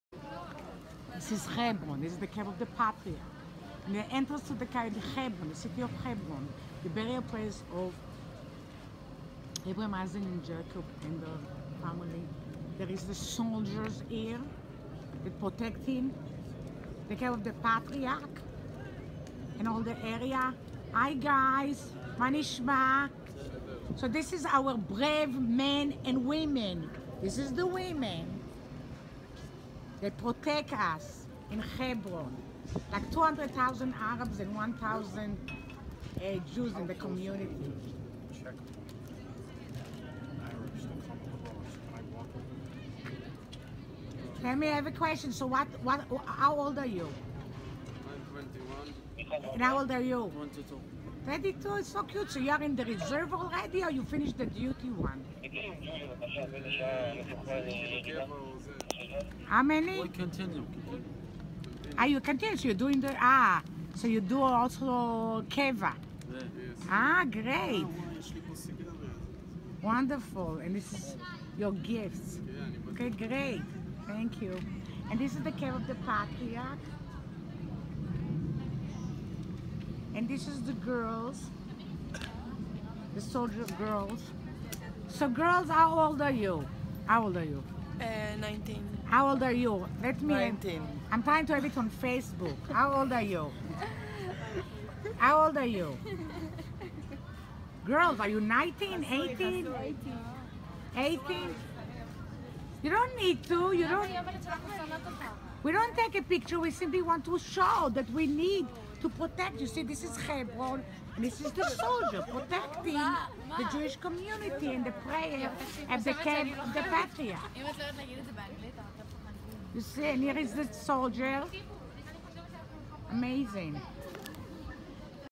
Israeli Soldiers Guarding the Cave of Patriarchs- Hebron
Israeli soldiers , young men and women soldiers,  guarding  at the entrance of The Tomb of Patriarchs in Hebron against Palestinian terrorism. The Israeli border police goal is to protect  all worshipers , both Jews and Muslims , with the goal of allowing freedom of worship  and religion  to both Jews and Muslims and citizens, residents,  and visitors. The Cave is a burial place of the Jewish biblical forefathers , Abraham, Issac and Jacob and their wives, Sarah, Rebecca, and Leah .

Despite the fact that the Cave is holy to Jews and Arabs ,   and under Israeli rule since 1967,  arabs  have been  allowed  to share the burial site and  to pray there,   Israeli soldiers  need to guards the site against repeated Arab Palestinian terrorist  attacks.

  Bombs are thrown at them and explosive devices are planted  around the Cave and knives attacks.  For example; on June 22, 2018 bomb was thrown at Israeli guards at the Tomb   thankfully with no injuries.
There has been 4000 years of jewish presence in Hebron  since Abraham, Issac and Jacob and king David. But under the British rule of the land of Israel,  In 1929, Hebron  was the site of some of the worst  of the rioting that killed 67 Jews  by the local Arabs. The clinic was looted and burned. During the Jordanian occupation of 1948-67 all Jews were barred from the City  . After the city was liberated, Jews returned back to the city   the jewish community  was rebuilt  . Immediately to respect Muslim rights of worship the Israeli government transfer administrative authority to the Waqf, the Muslim religious Authority despite the fact that under Muslim Arab rule for 19 years ( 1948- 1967) Jews were barred form entering East Jerusalem and the entire West Bank including praying at  the Western wall, Rachel Tomb, Joseph Tomb the Cave of the Patriarchs.

Over 300,000 people visit Ma'arat HaMachpelah - Cave pf the Patriarchs  annually.

to see more work from Shoula Romano Horing!